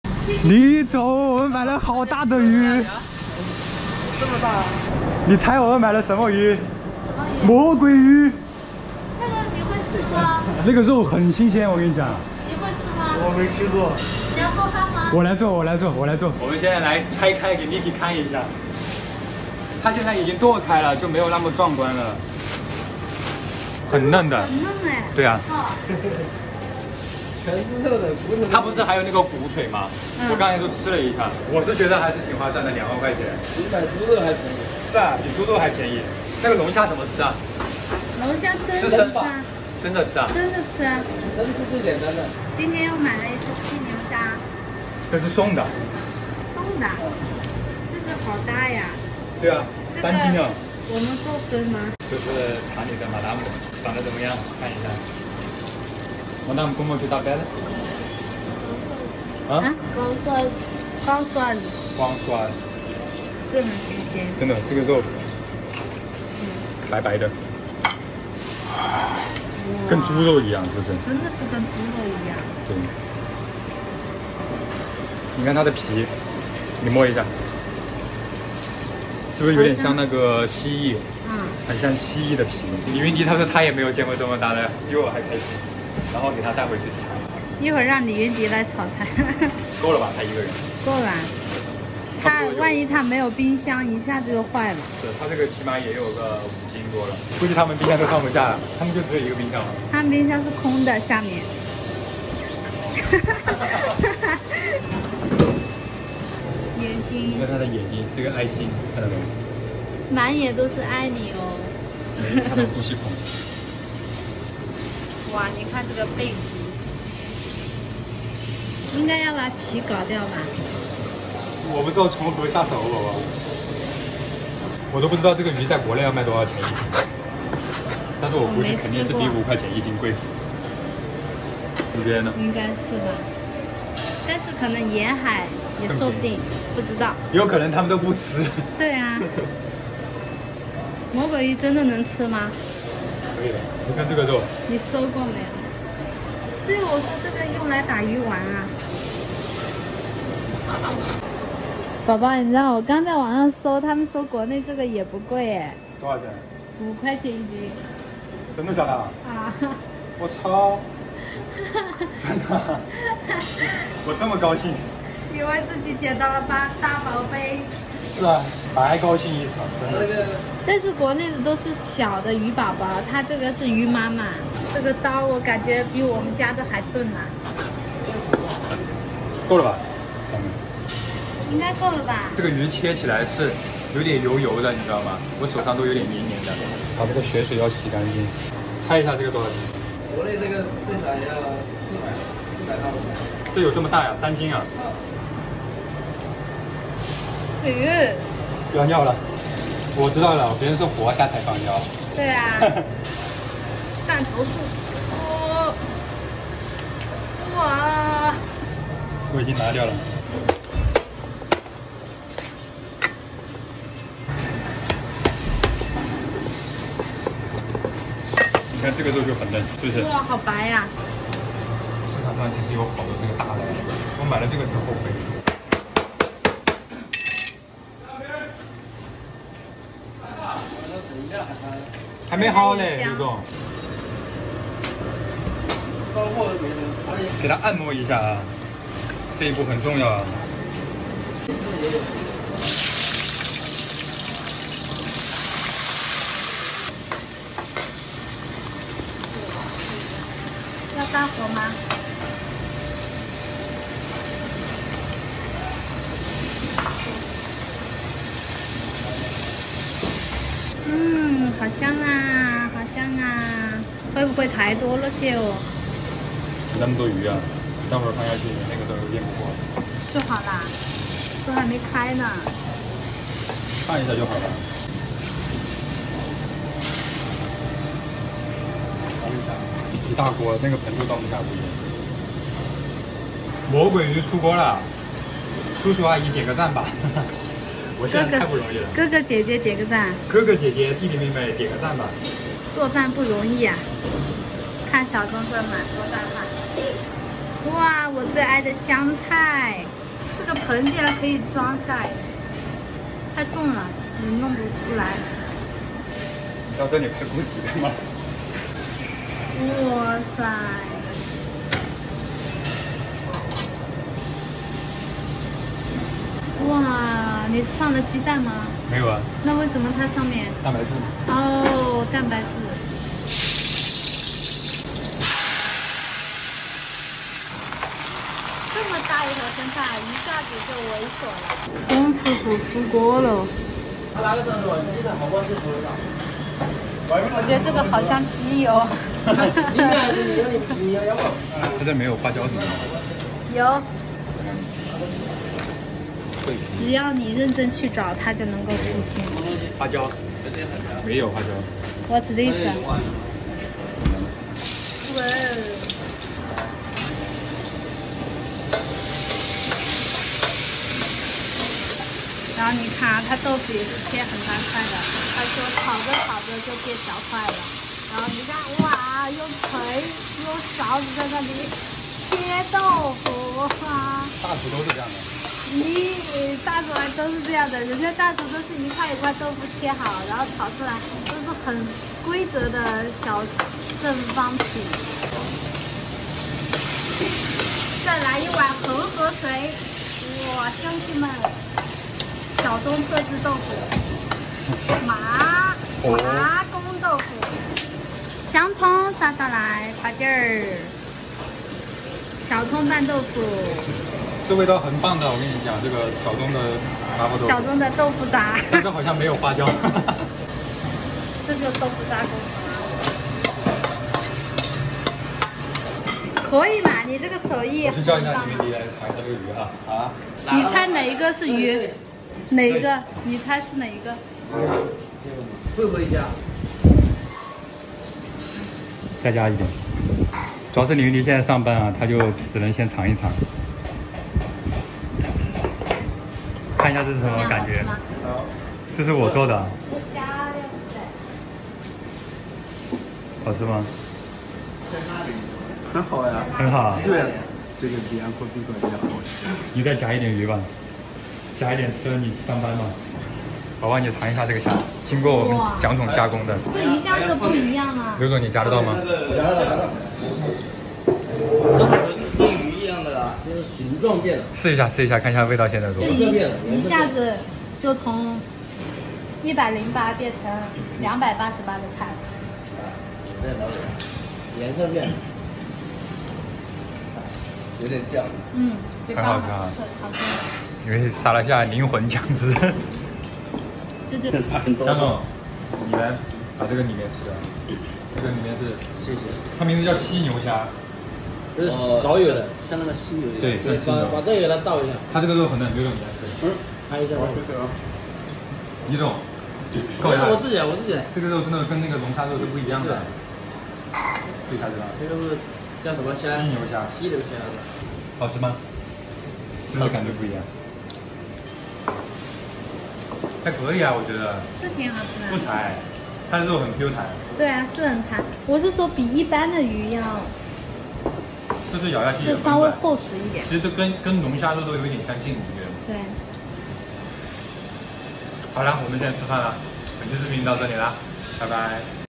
200块在非洲买了一条40斤魔鬼鱼，小钟做了水煮鱼，味道太鲜美了
接上期视频，200块在非洲买了一条40斤魔鬼鱼，今天小钟在朋友家做了水煮鱼，味道很鲜美，云迪很爱吃。非洲小钟